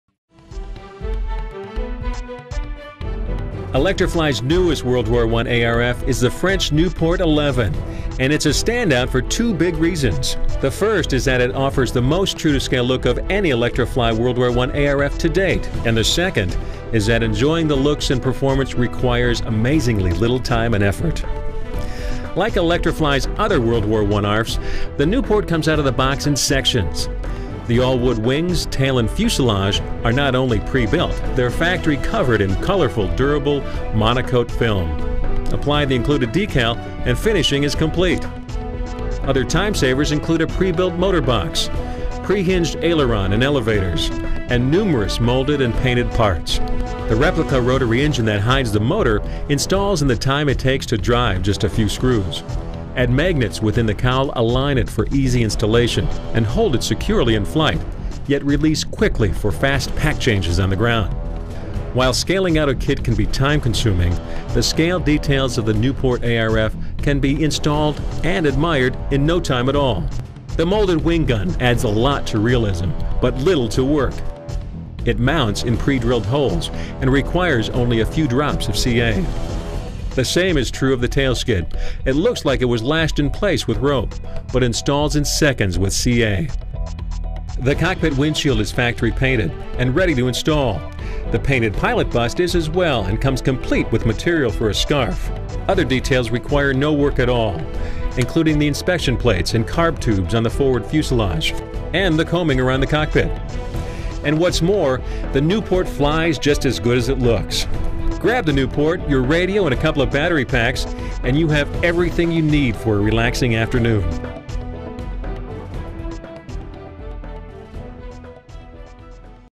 Spotlight: ElectriFly Nieuport XI EP WWI Parkflyer ARF
With the 35.5 span Nieuport, ElectriFly reduces assembly time to just 4-6 hours while delivering its finest scale WWI looks to date. Details include coaming and a windshield for the cockpit; a painted pilot figure complete with jaunty fabric scarf; authentically detailed Lewis gun on the top wing; replica rotary engine in the nose; and a striking, 4-color MonoKote® trim scheme based on the French Escadrille 48.